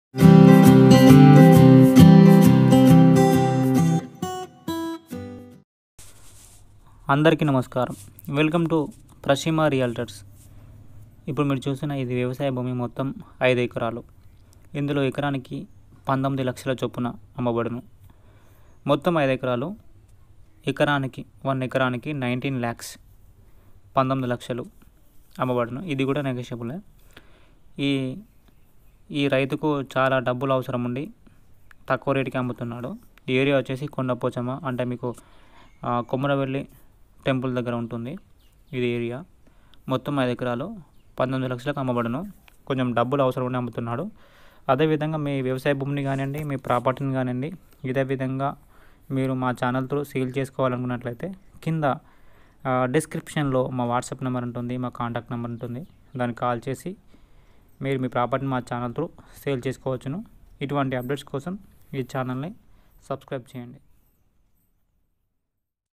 ₹19 lakh 1 acre for sale kondapochamma komuravelli || 19lakh 1acre || Prashi Maa Realtors
This agriculture land total 5acres 1acre 19lakh for sale area kondapochamma komuravelli

---**- SOLD OUT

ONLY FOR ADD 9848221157